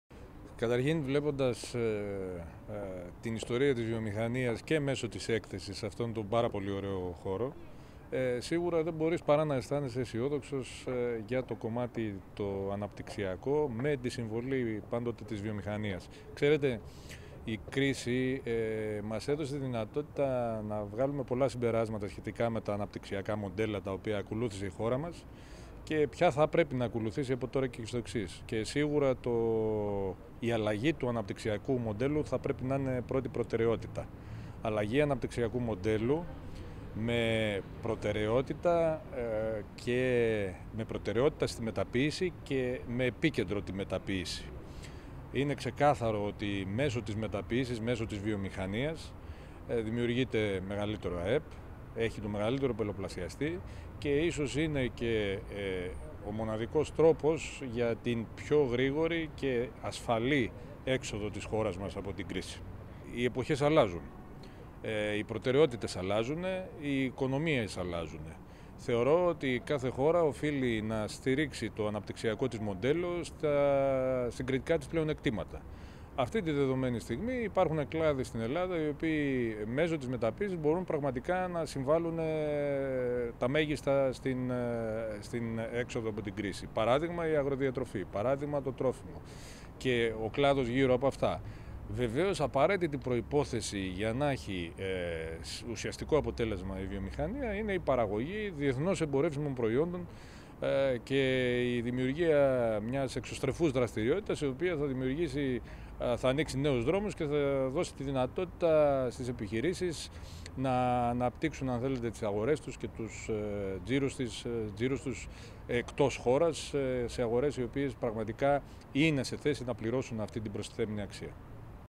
O Aθανάσιος Σαββάκης για την ελληνική βιομηχανία
Στο πλαίσιο της εκδήλωσης "Ελληνική Βιομηχανία: Αλήθειες & Μύθοι" της economia Publishing που πραγματοποιήθηκε στις 13 Μαρτίου 2018 στο INNOVATHENS, o Πρόεδρος του ΣΒΒΕ, Αθανάσιος Σαββάκης, μιλάει για τα κλειδιά της ανάπτυξης της ελληνικής βιομηχανίας.